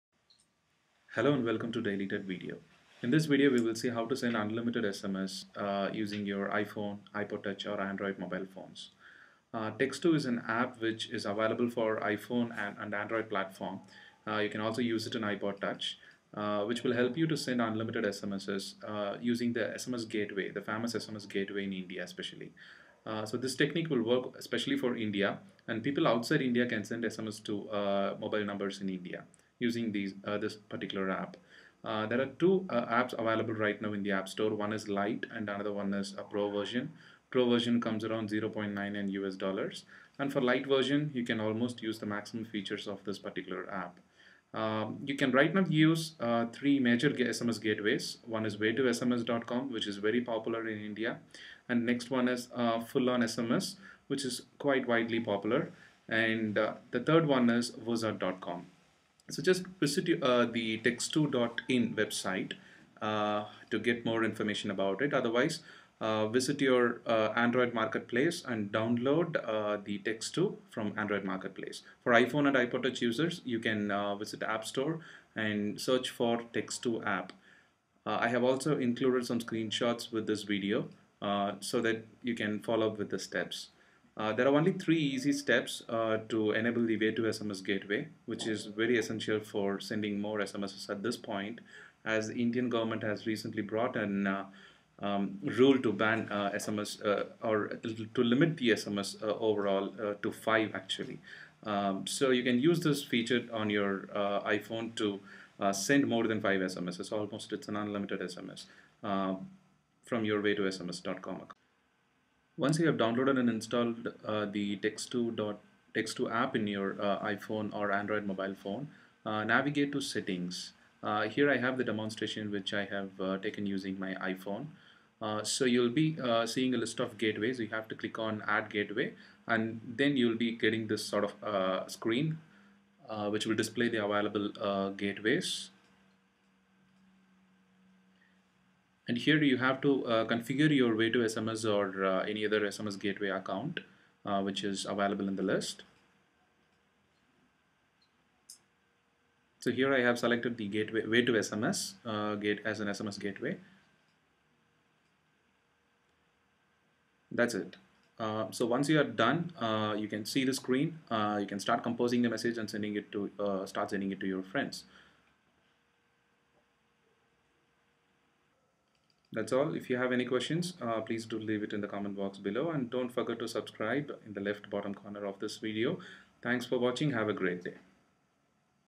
Send More than 5 SMS Per Day [HOW TO]
Textoo App for iPhone, iPod Touch and Android mobile phones can let you send unlimited SMS messages without any limit using your Way2SMS Gateway and few other SMS Gateways are supported as well.